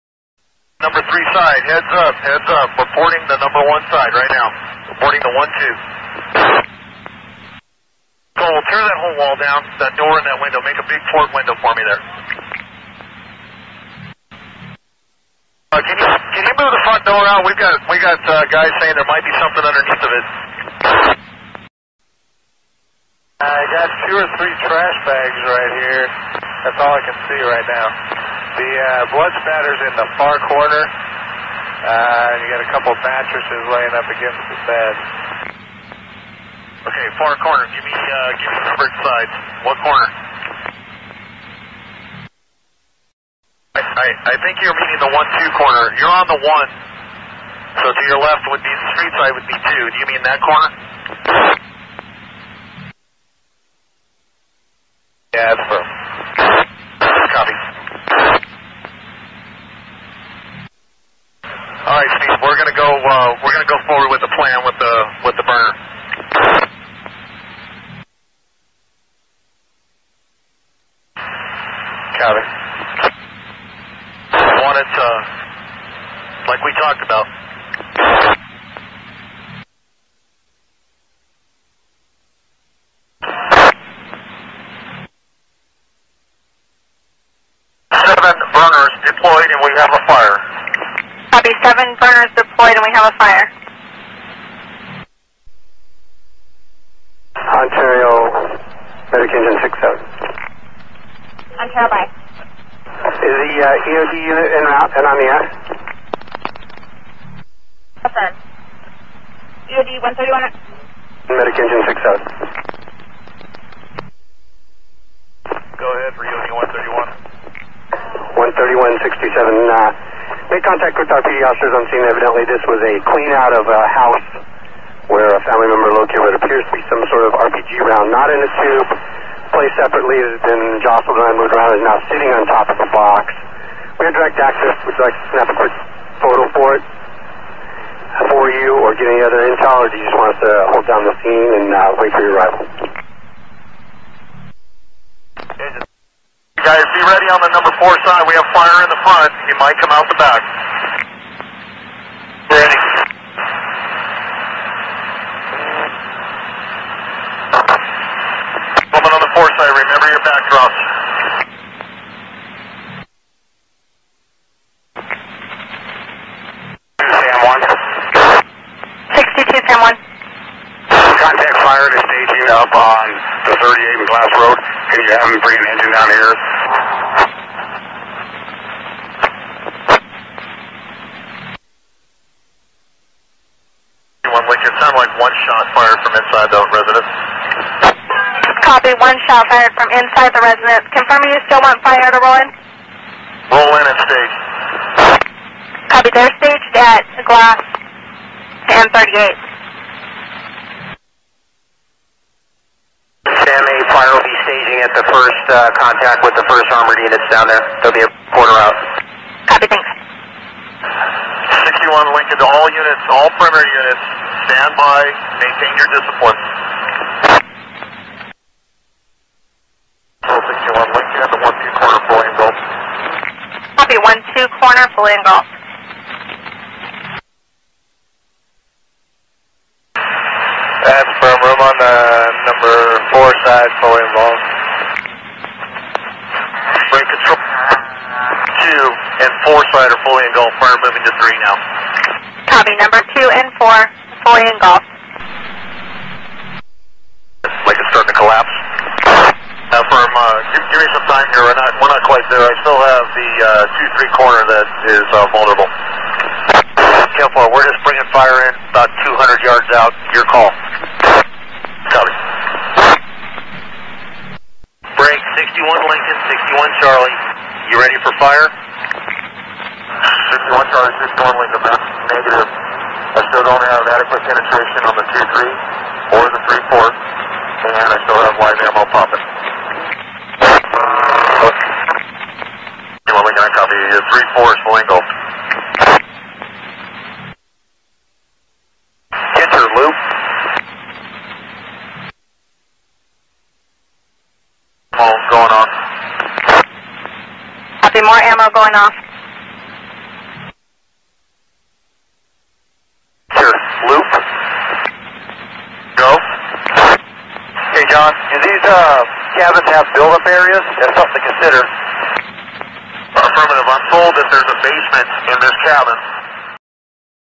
Christopher Dorner Shootout - Police Radio Scanner - Police lynched Dorner! 13.02.2013
As you can see on tv it says "Dorner is dead".
The truth is, police just tried to lynch Dorner! They burned down the house! Your Choice: Thumbs up for Victim! Thumbs down for murderer!

"He just came out of the snow at me with his gun at my head. He said, 'I don't want to hurt you. Just get out of the car and start walking.'"

The cabin caught fire after police tossed smoke devices inside, a law enforcement source told CNN.  (smoke devices...uhh ok... LOL)